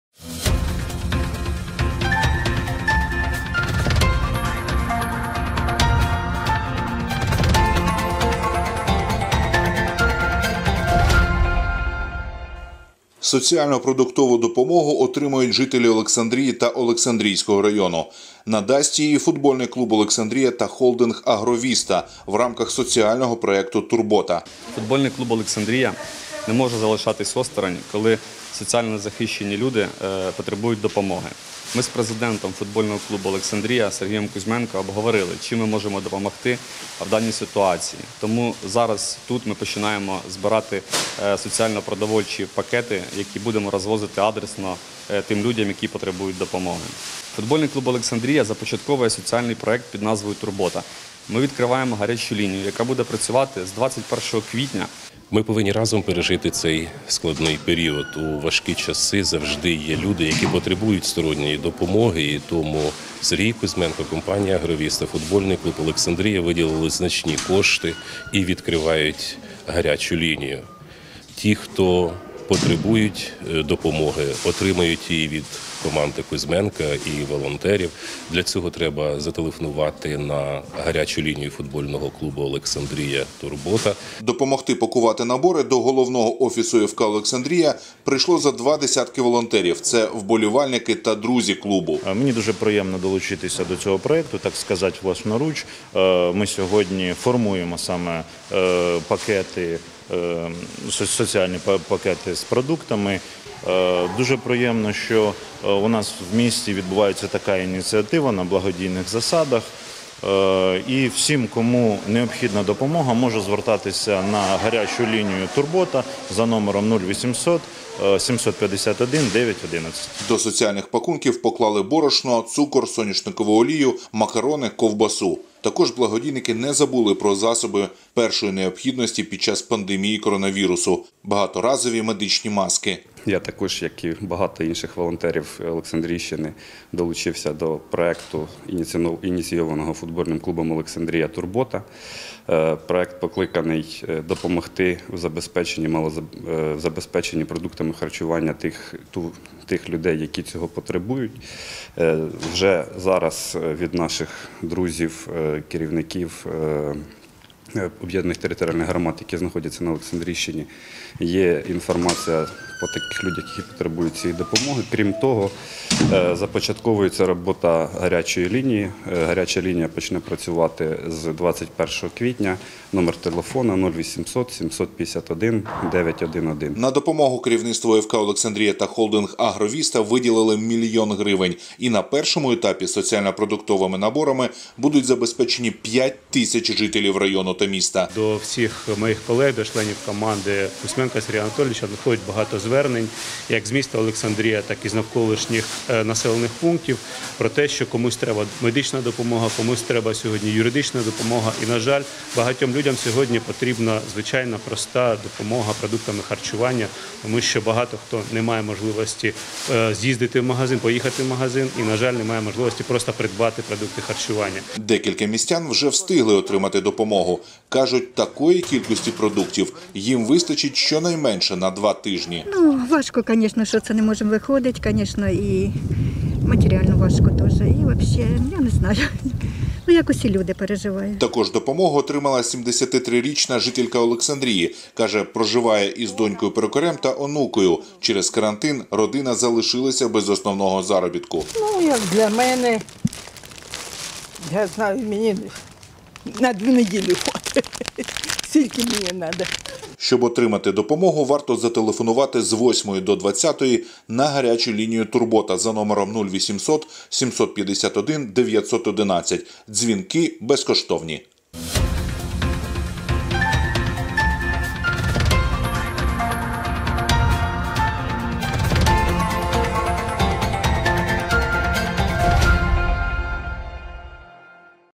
Соціально-продуктову допомогу отримають жителі Олександрії та Олександрійського району
Надасть її футбольний клуб «Олександрія» та холдинг «Агровіста» в рамках соціального проекту «Турбота».